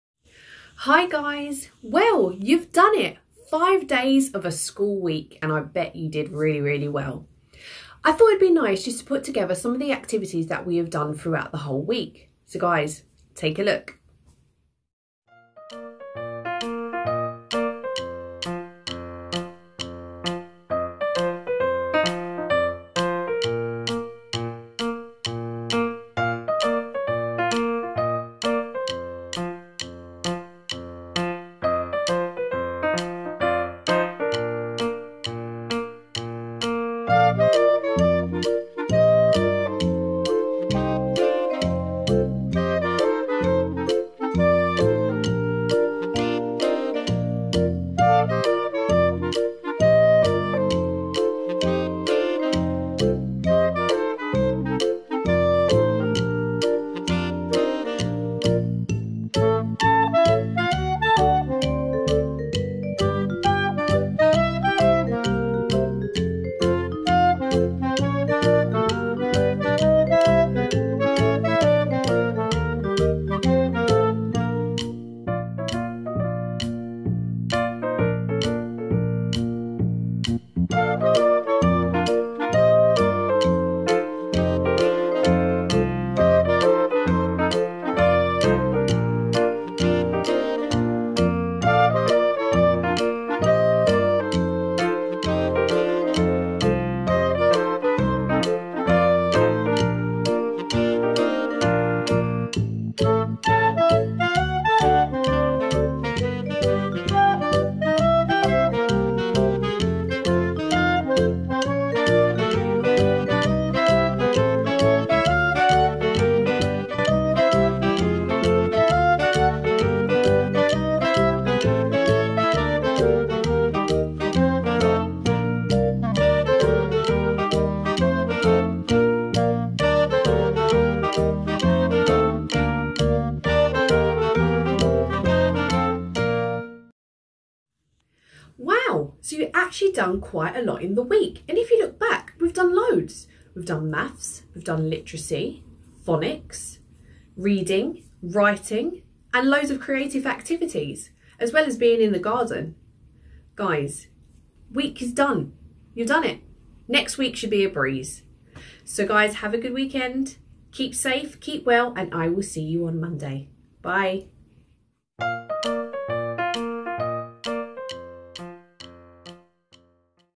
We did it! 1 week of home schooling complete!
hi guys, we finally crossed the finish line and completed our first week of home schooling!